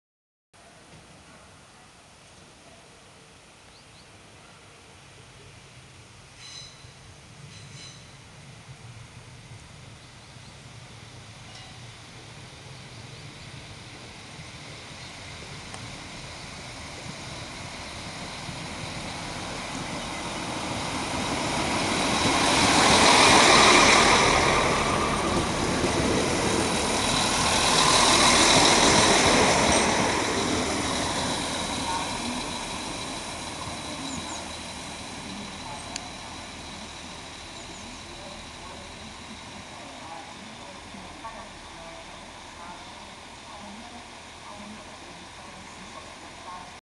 Příjezd vlaku Os 7082 (ČD 844.005) - Klášterec nad Ohří, 28. 10. 2013
Tento vlak slouží spíše jako přesun soupravy z Chomutova do Klášterce n.O. a to pouze o víkendech a svátcích, protože je potřeba, aby něco odjelo v 9.07 do Mostu v taktu a to "něco" v Klášterci chybí. A protože tento vlak vyjíždí z Chomutova jen o 8 minut později než předchozí Os 7050 a nestaví nikde, nelze se divit, že s ním nikdo nejel.

Chomutov (8.42) - Klášterec nad Ohří (8.58)
Natočeno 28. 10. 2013 v 8.59